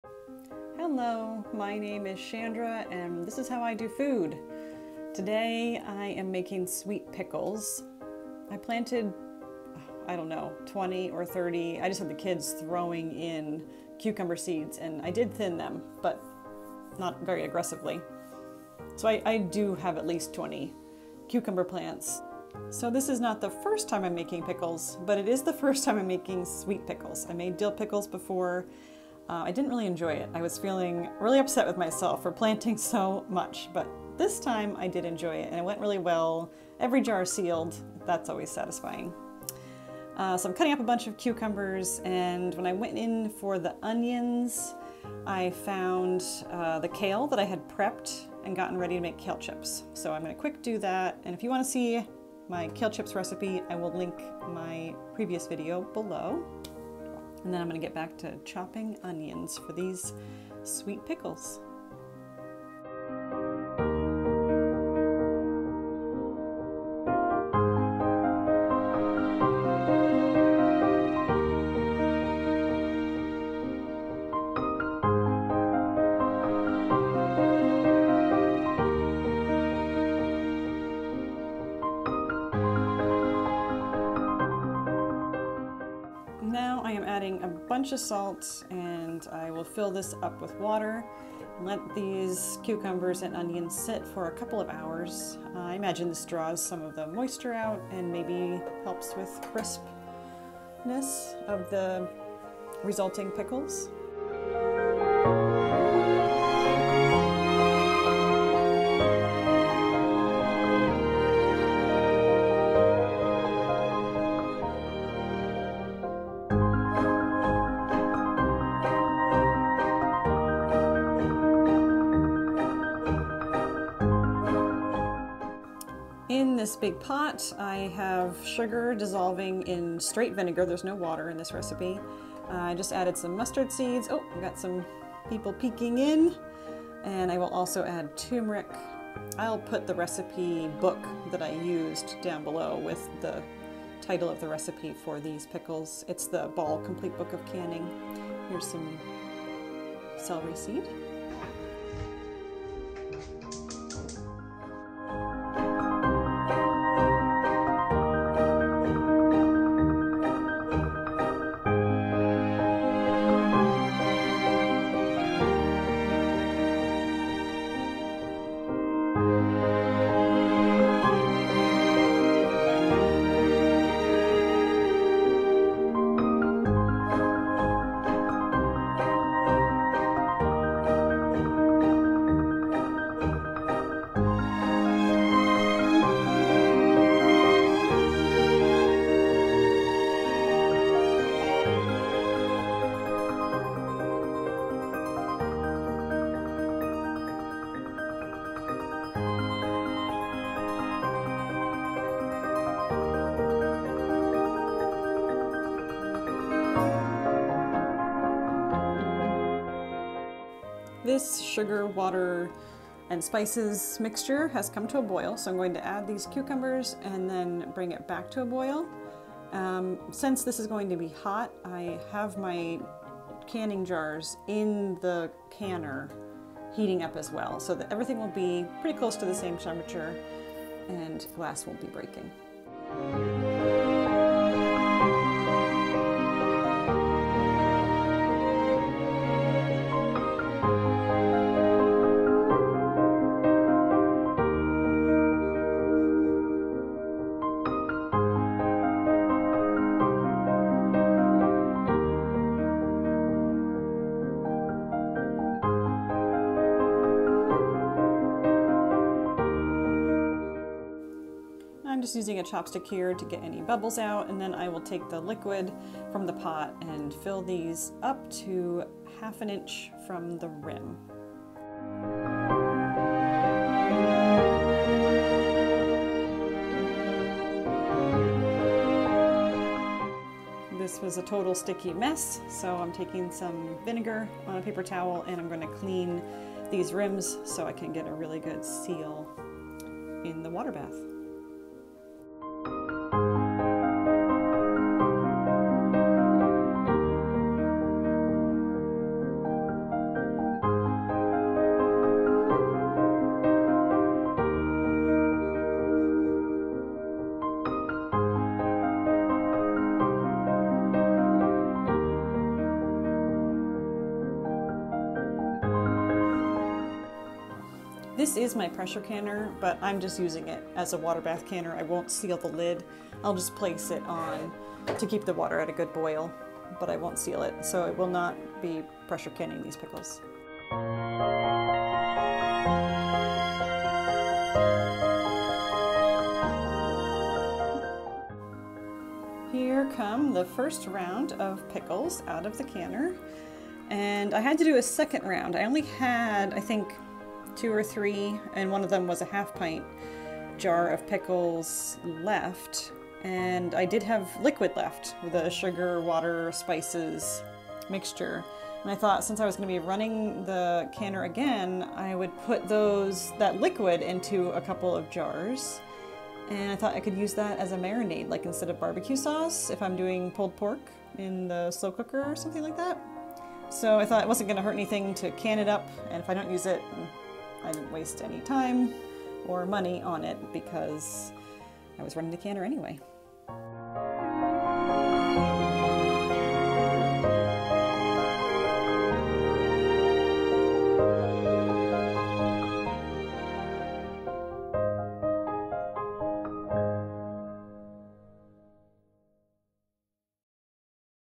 So many cucumbers! Must make pickles! These are sweet pickles, or bread and butter.
I may have planted too many cucumbers, but now my storage cabinets and refrigerator are chock-full of pickles.  These latest ones are the ‘Traditional Bread and Butter Pickles’ from the Ball Complete Book of Home Preserving.